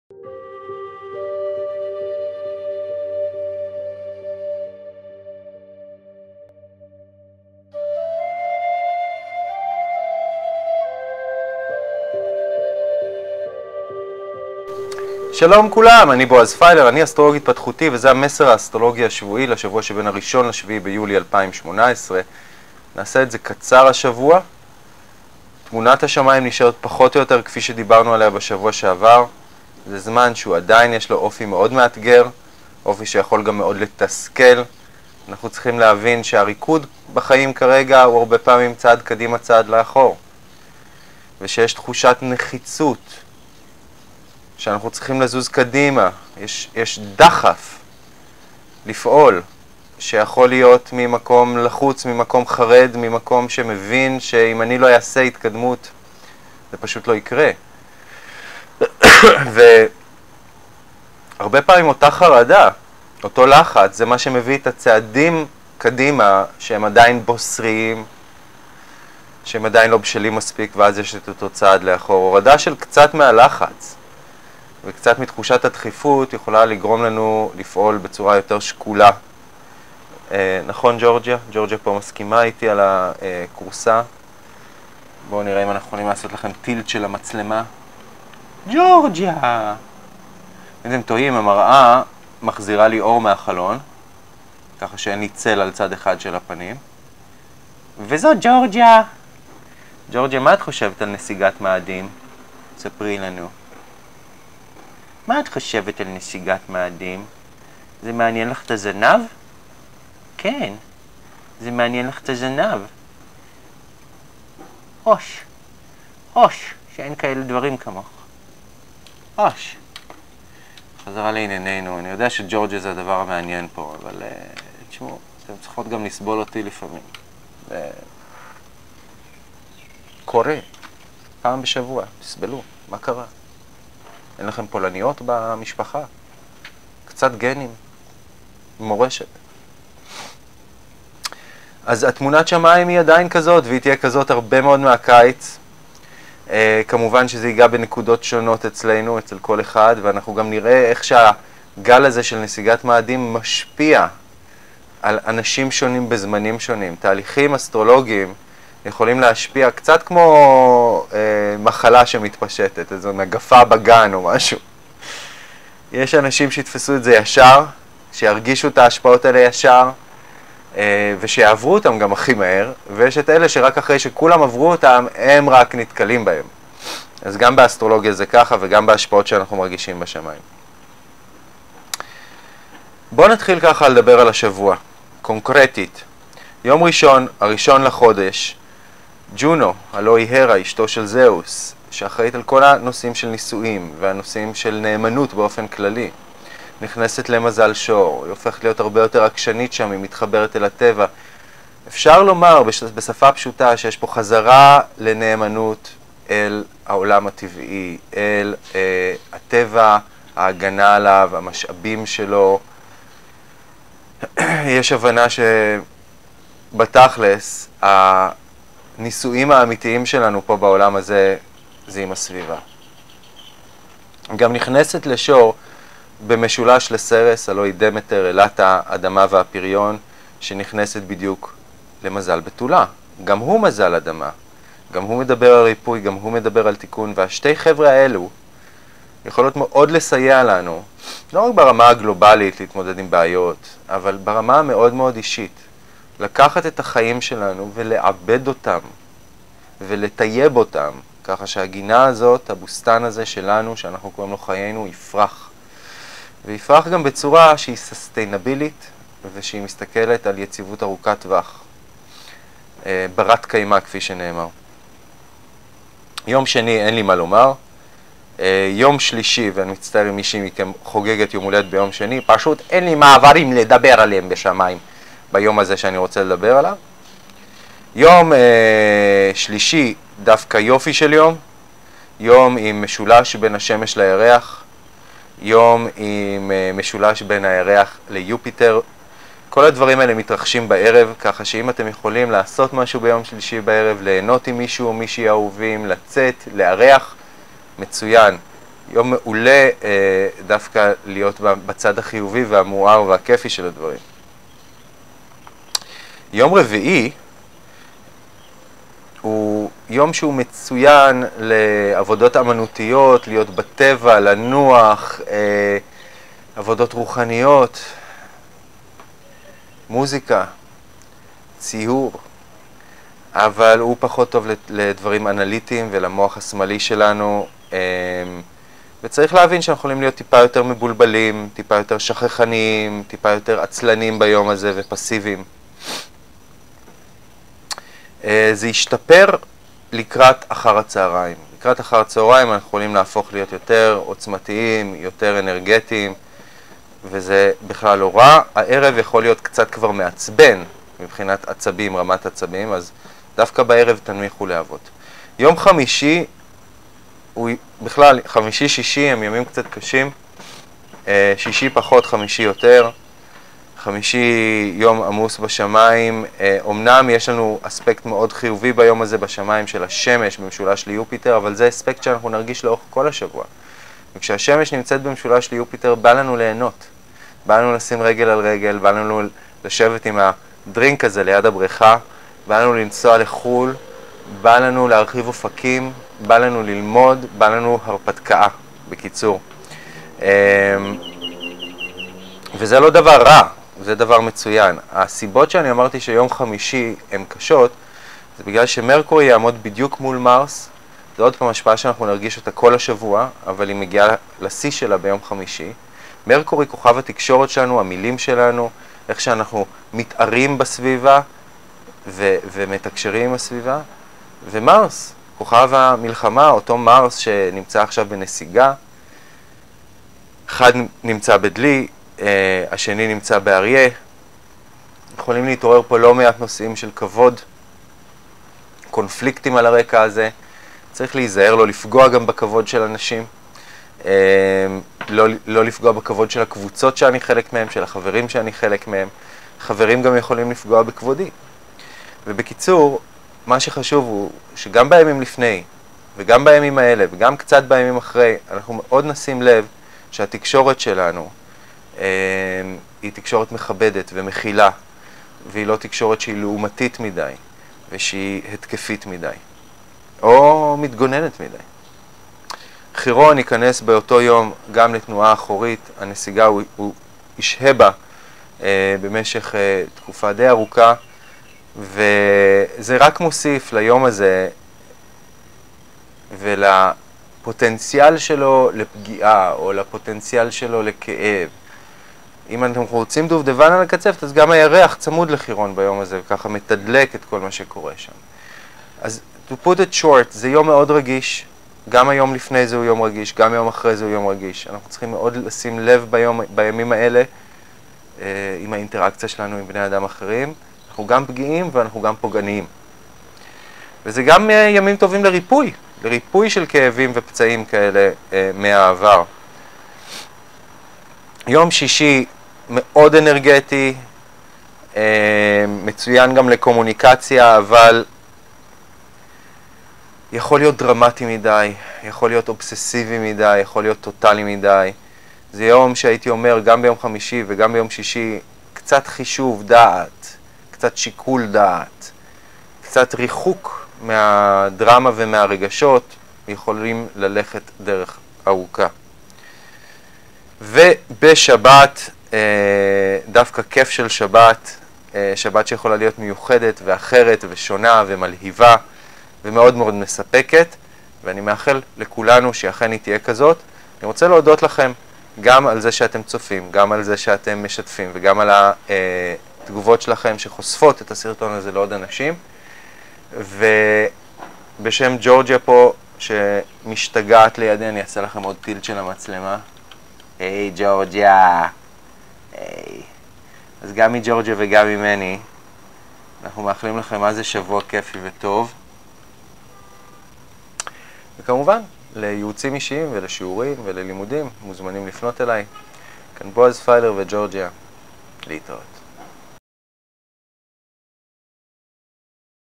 ג'ונו בשור - המסר האסטרולוגי השבועי עם בועז פיילר 1-7 ביולי 2018
האסטרולוג ההתפתחותי בועז פיילר עם תחזית קצרה וקולעת השזורה במסרים מהלב. מטרת הסרטון להוות כלי שיכול לסייע לך להעביר את השבוע בצורה יותר מוצלחת. תודה רבה על הצפייה, התגובות והשיתופים!

זה המקום שלכם למצוא תחזית אסטרולוגית רוחנית (הורוסקופ) המתאימה לכל המזלות.